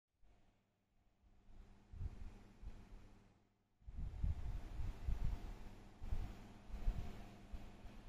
when ac is on 😂😆 😆😅
Stella The Cat is playing while having Fun.